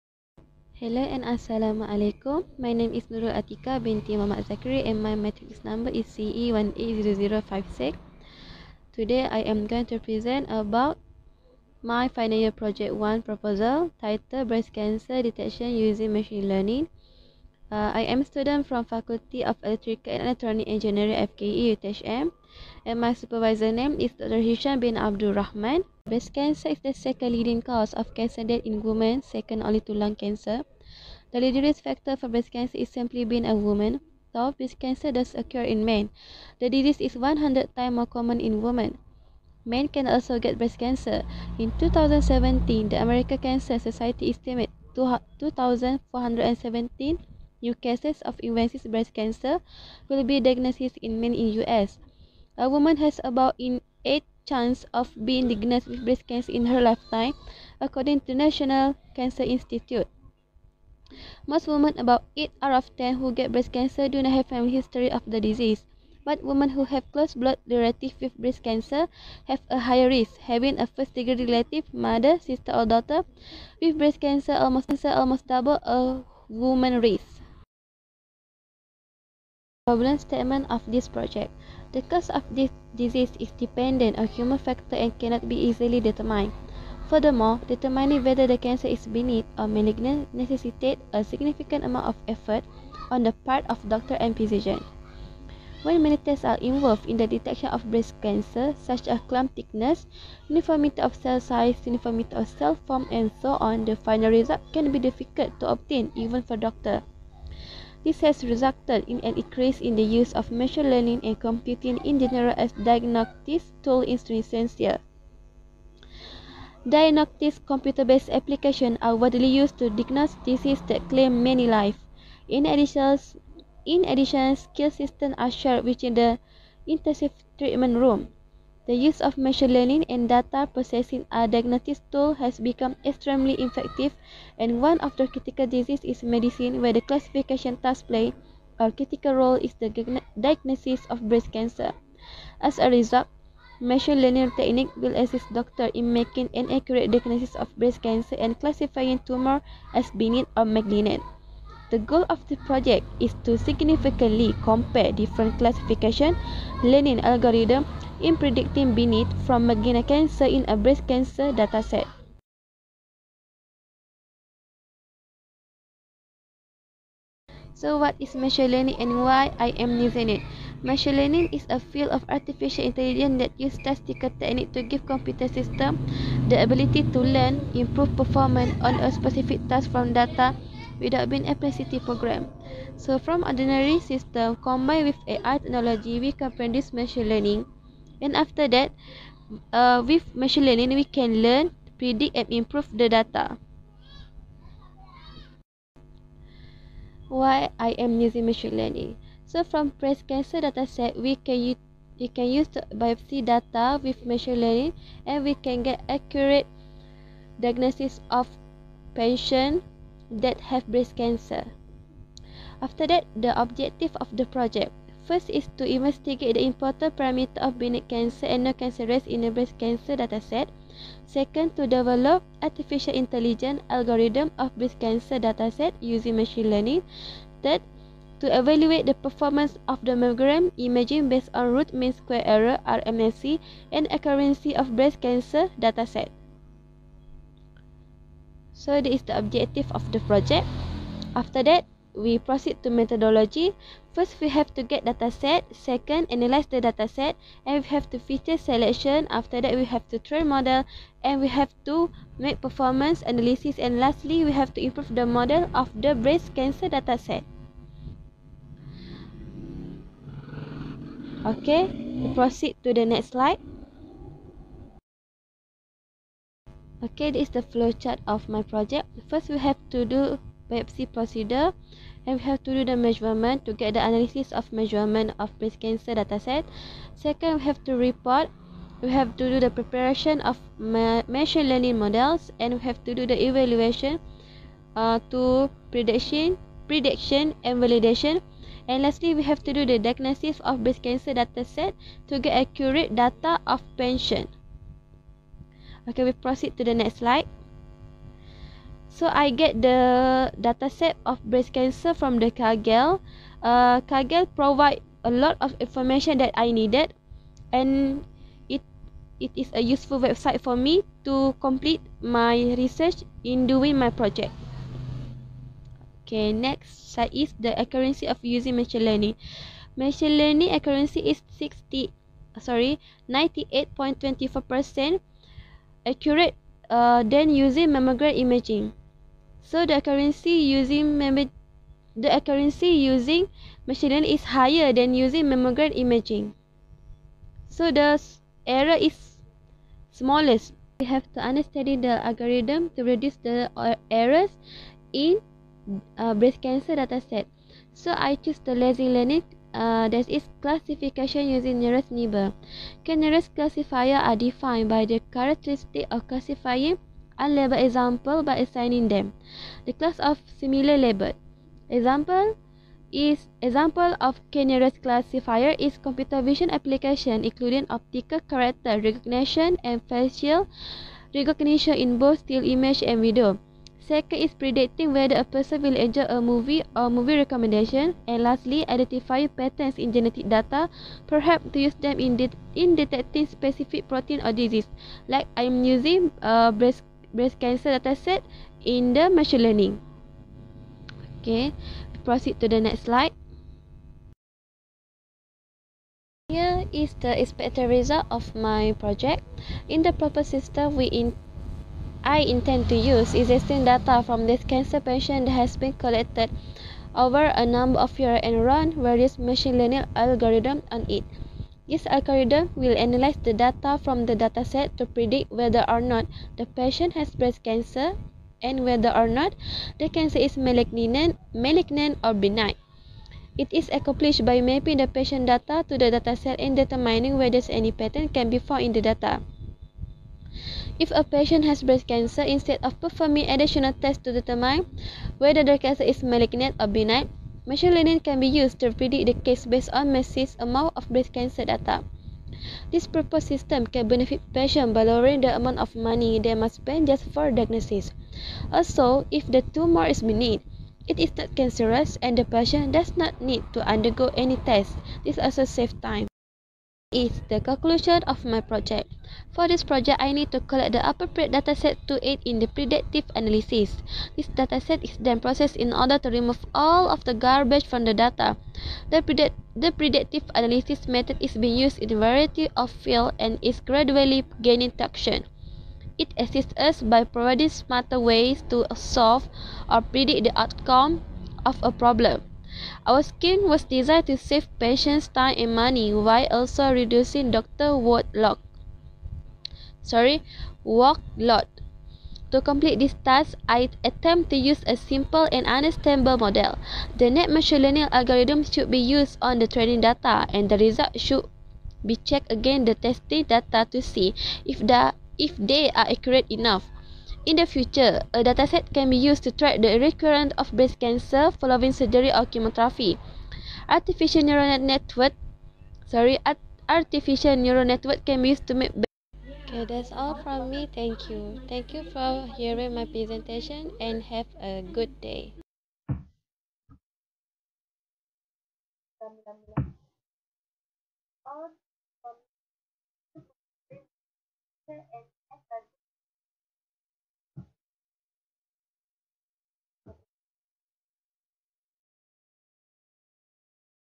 Breast Cancer Detection Using Machine Learning.
Hello there. I'm Nurul Atikah from Uthm, currently doing my presentation on my final year project (Fyp1). Please give comment about my video presentation in order to improve better performance. Thank you for your time and have a good day.